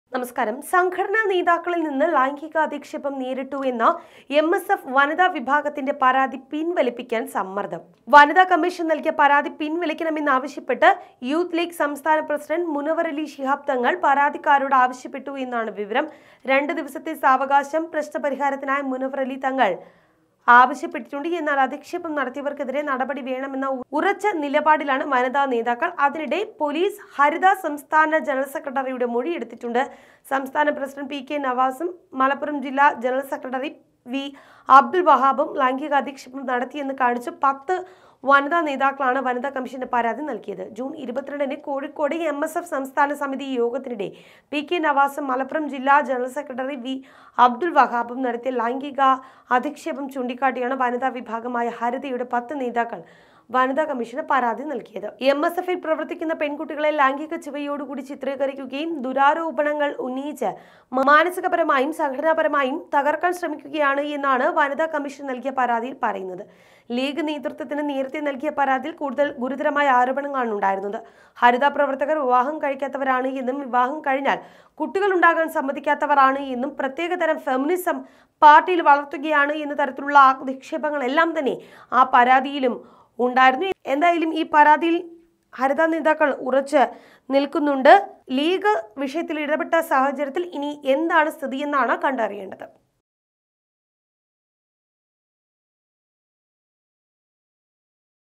ലൈംഗിക അധിക്ഷേപ പരാതി പിന്‍വലിക്കണമെന്ന് മുനവറലി ശിഹാബ് തങ്ങള്‍ l MSF Haritha
അധിക്ഷേപം നേരിടേണ്ടി വന്നെന്ന എംഎസ്എഫ് വനിതാ വിഭാഗത്തിന്റെ പരാതി പിന്‍വലിപ്പിക്കാന്‍ സമ്മര്‍ദ്ദം. വനിതാ കമ്മീഷന് നല്‍കിയ പരാതി പിന്‍വലിക്കാന്‍ സമ്മര്‍ദ്ദം.. യൂത്ത് ലീഗ് സംസ്ഥാന പ്രസിഡന്റ് മുനവറലി ശിഹാബ് തങ്ങള്‍ പരാതിക്കാരോട് ആവശ്യപ്പെട്ടു